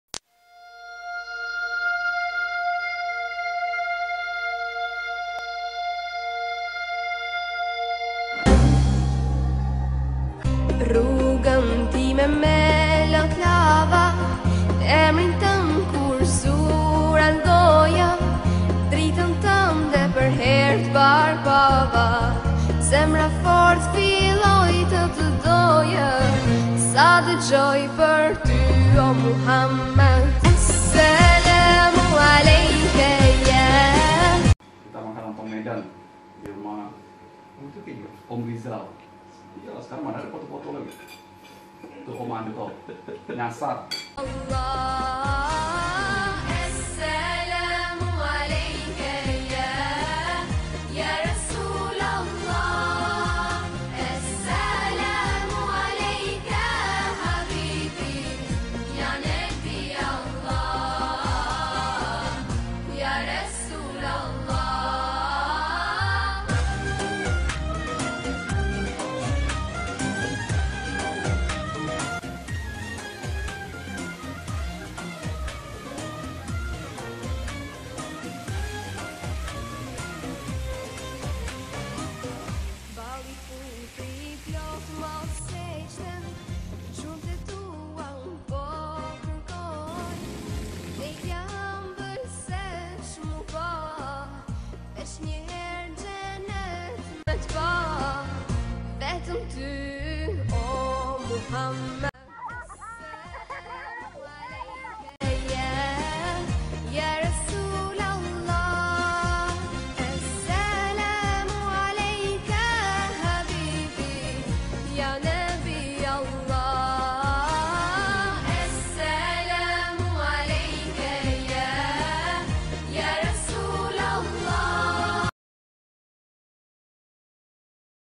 Berkunjung ke Rumah Rizal
Silaturahmi antara sesama sahabat.
Dulu kami bertiga 1 kosan, pernah satu kamar masing-masing dan merantau juga ke Jakarta bersama2.